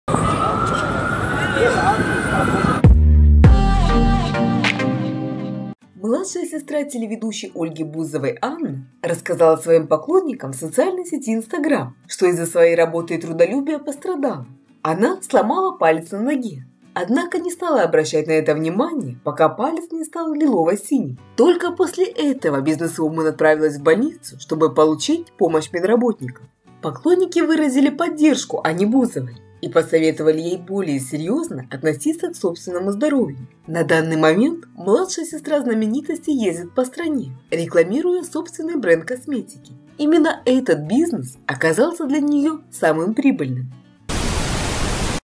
С первым переломом меня» Бузова получила травму
Младшая сестра певицы, телеведущей и звезды проекта "Дом-2" Ольги Бузовой Анна, как и многие, старается заранее подготовиться к новогодним праздникам. Тем не менее, трудолюбие может сыграть с девушкой плохую шутку, ведь она уже сломала палец на ноге.